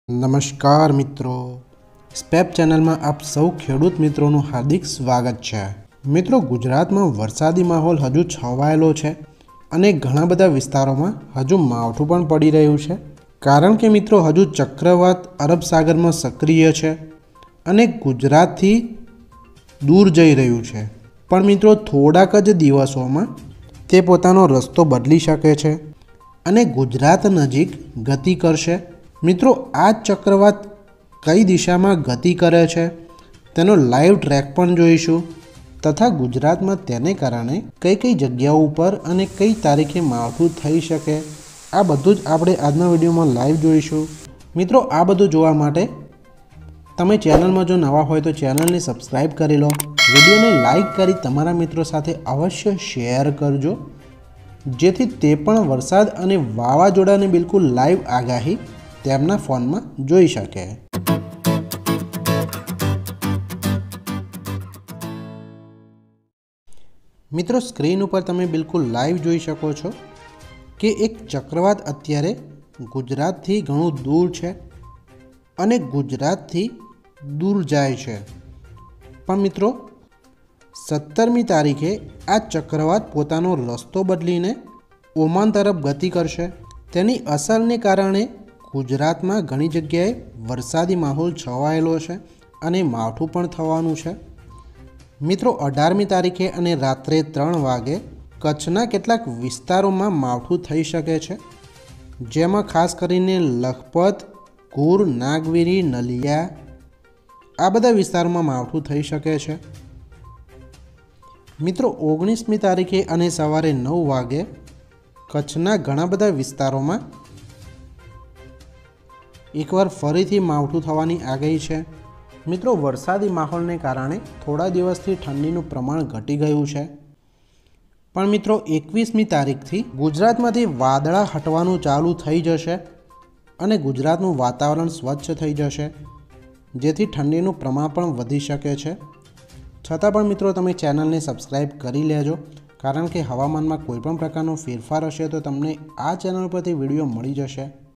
સાવધાનગુજરાત! વાવજોડા રસ્તો બદલ્યો,19તારીખસુધી માવઠું/weather forecast in gujarat, weathertv, spep
સાવધાનગુજરાત!19તારીખસુધીમાવઠુંભુક્કાબોલાવશે,ધુમ્મસઆગાહી/weather forecast in gujarat, weathertv, spep